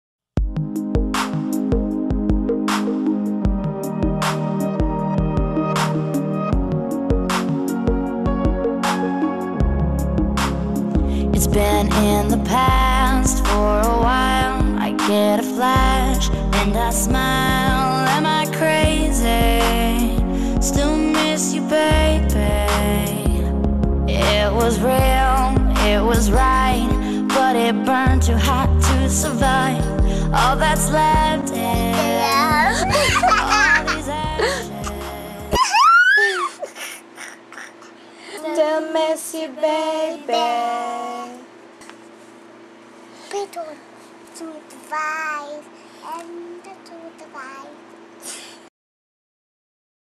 FRIENDSANDFAMILY- *Cute~ Lizzy*
Cute 4 year old Khalisah making her first little music video and loves it! She loves seeing the finished product. We both had alot of fun making it. We always create something in our time together. Either a drawing, a video or a photoshoot.. She is a very talented little girl!